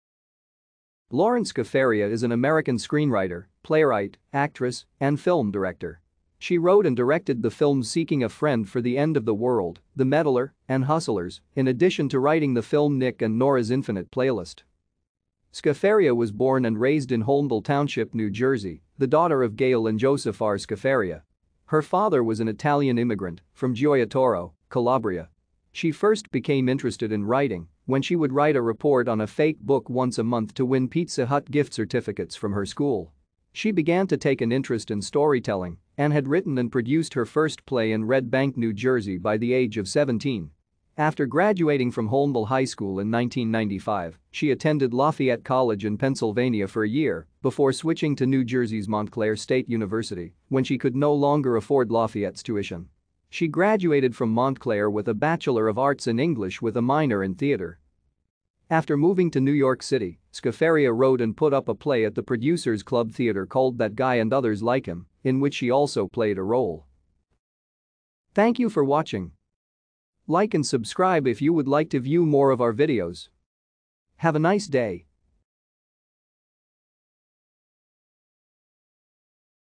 Lorene Scafaria
Lorene Scafaria  is an American screenwriter, playwright, actress, and  director. She wrote and directed the s Seeking a Friend for the End of the World, The Meddler, and Hustlers, in addition to writing the  Nick & Norah's Infinite Playlist.